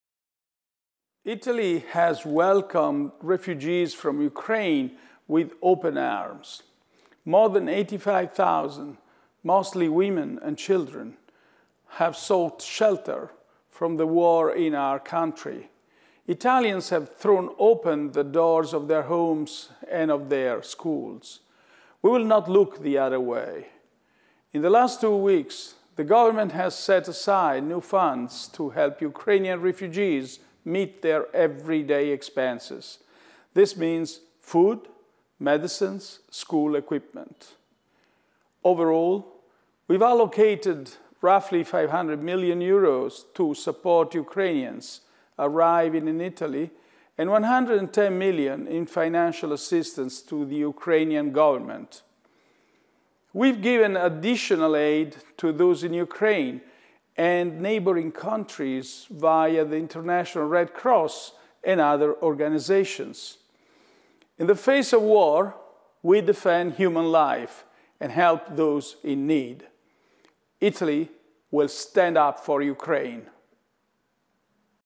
Stand Up For Ukraine, il messaggio di Draghi
(Agenzia Vista) - Roma, 9 Aprile 2022 - "L'Italia ha accolto a braccia aperte i profughi dall'Ucraina. Più di 85.000, per lo più donne e bambini, hanno cercato riparo dalla guerra nel nostro Paese. Gli italiani hanno spalancato le porte delle loro case e delle loro scuole. Non guarderemo dall'altra parte. Nelle ultime due settimane, il Governo ha stanziato nuovi fondi per aiutare i rifugiati ucraini a far fronte alle loro spese quotidiane. Ciò significa cibo, medicine, materiale scolastico. Complessivamente, abbiamo stanziato circa 500 milioni di euro per sostenere gli ucraini che arrivano in Italia e 110 milioni per l'assistenza finanziaria al governo ucraino. Abbiamo fornito ulteriore aiuto a coloro che si trovano in Ucraina e nei paesi limitrofi tramite la Croce Rossa Internazionale e altre organizzazioni. Di fronte alla guerra difendiamo la vita umana e aiutiamo i bisognosi. L'Italia difenderà l'Ucraina". Il messaggio di Draghi per l'iniziativa Stand Up For Ukraine. / Palazzo Chigi